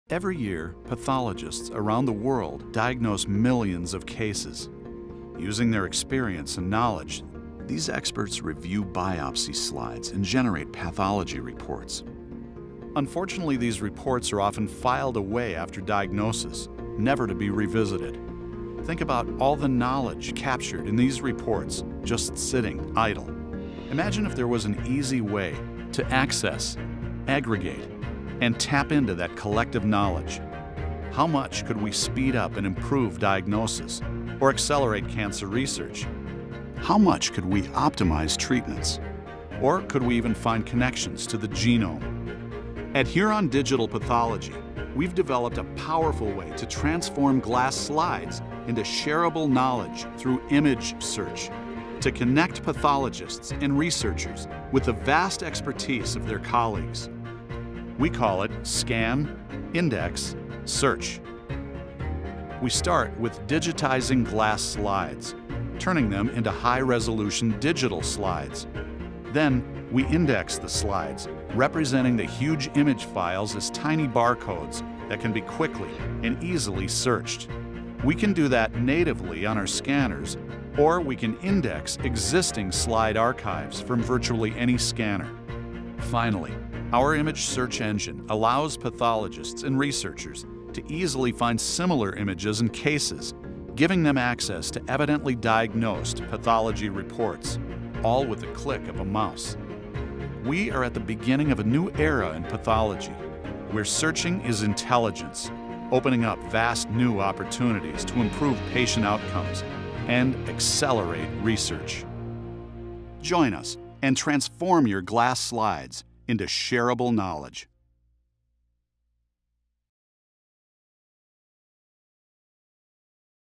Huron Scan Index Search
We are on a mission to transform glass slides into shareable knowledge. Our Scan, Index, and Search solution for pathology combines award-winning whole slide imaging hardware with powerful image search technology to connect pathologists, researchers and educators with the expertise of their colleagues to help speed up diagnosis and accelerate disease research.